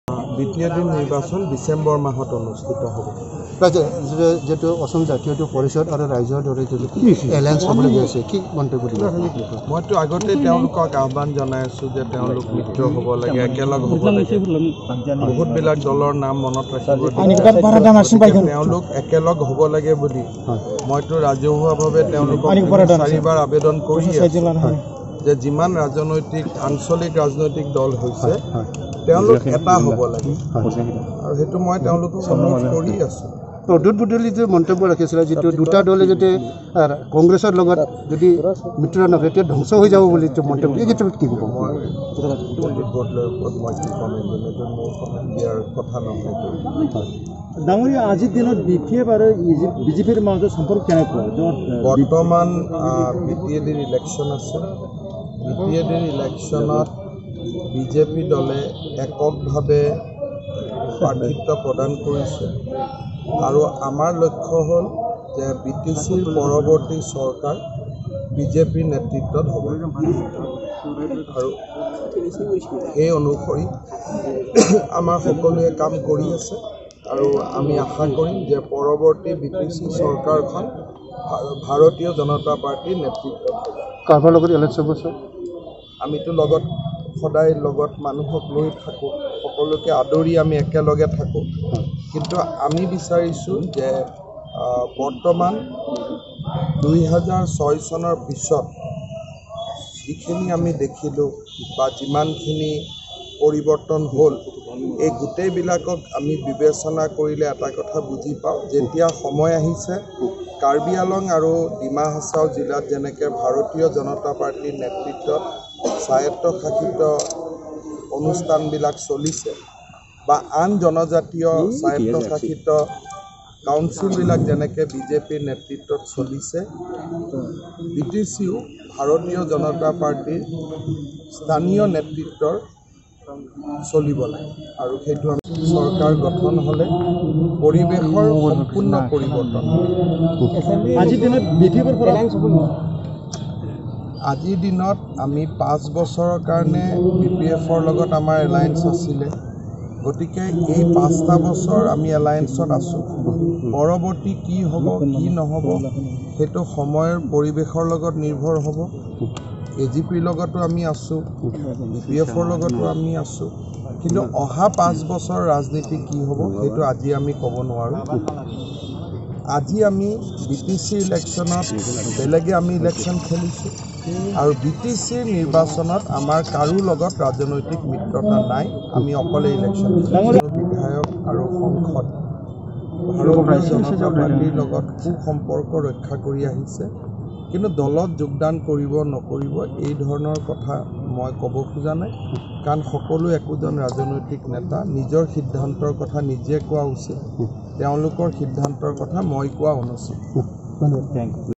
btc election himantabiswasarma btr
কোকৰাঝাৰত উপস্থিত হৈ মন্ত্ৰী হিমন্ত বিশ্ব শৰ্মাৰ ঘোষণা ।
বিটিচিৰ নিৰ্বাচন অনুষ্ঠিত হব ডিচেম্বৰ মাহত ।
বিটিচিৰ চৰকাৰ বিজেপিৰ নেৰ্তৃত্বতে গঠন হব ।
কাৰ্বি আংলং , ডিমা হাছাওৰ দৰে বিটিচিতো বিজেপিৰ নেৰ্তৃত্বতে চৰকাৰ গঠন হব ।
পাঁচ বছৰৰ বাবে বিপিএফৰ সতে মিত্ৰতা হৈছিল , পৰবৰ্তী পাঁচ বছৰত সময় আৰু পৰিৱেশৰ ওপৰত নিৰ্ভৰ কৰিব । বিটিচিৰ নিৰ্বাচনত অকলশৰীয়াকৈ অংশগ্ৰহণ কৰিছে বিজেপিয়ে ।
বিপিএফৰ একাংশ নেতাই দলত যোগদান কৰিব নে নাই নাজানো কিন্তু যোগাযোগ আছে , তেওঁলোকে নিজৰ সিদ্ধান্ত নিজে ঘোষণা কৰা উচিত ।